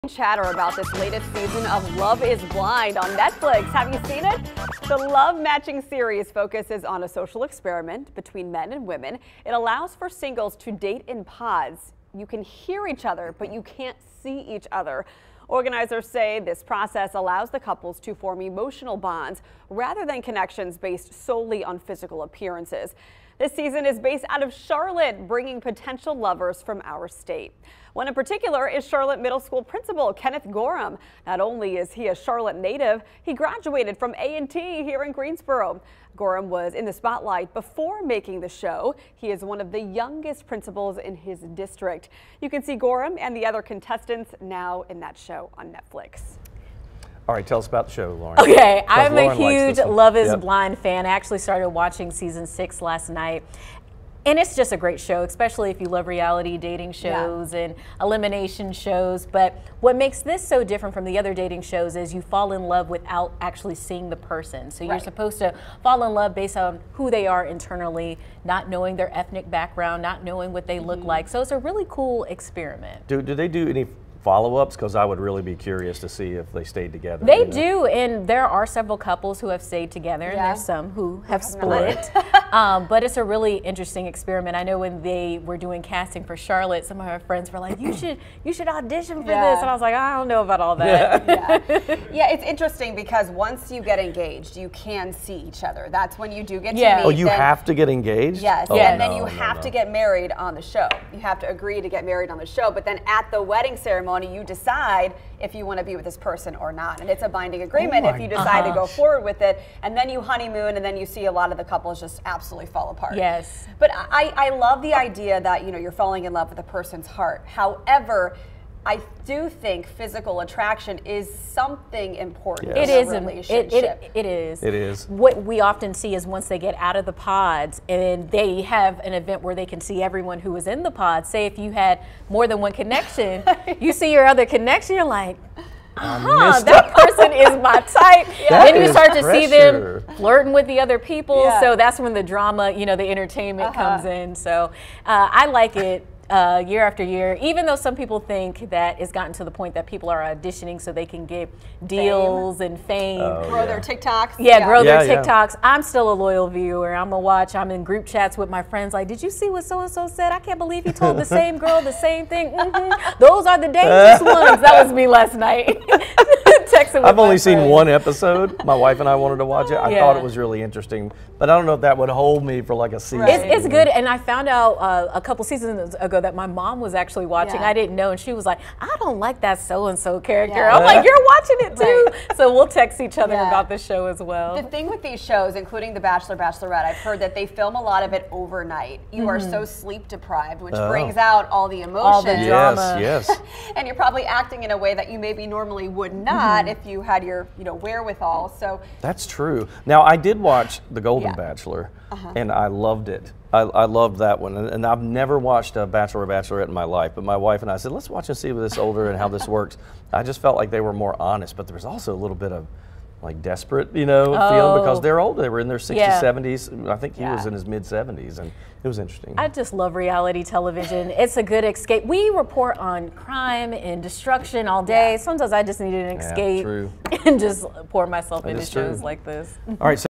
NC A&T alum apart of Love is Blind cast
Charlotte native and Aggie alum Kenneth Gorham is a part of the season 6 cast of Love is Blind on Netflix. He is also the youngest principal in his school district.